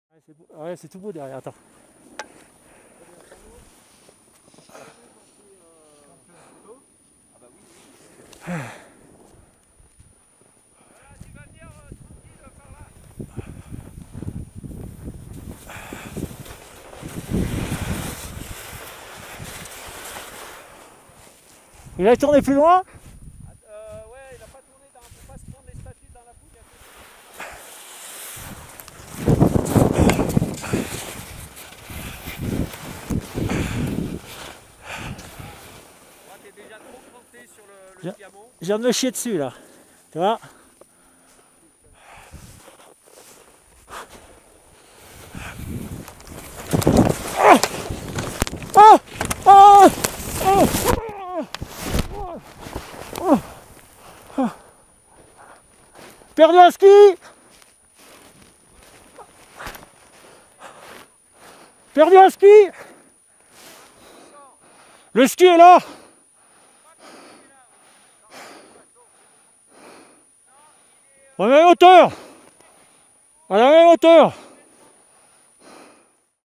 Chamonix - Vallée Blanche - Grand Envers du Plan (footage)
Pour passer une rimaye il y a plusieurs solutions possibles. j'ai "choisi" la plus rapide mais pas la plus élégante (on en peut pas tout avoir). Pou rmon premier Grand Envers du Plan j'ai été bien baptisé.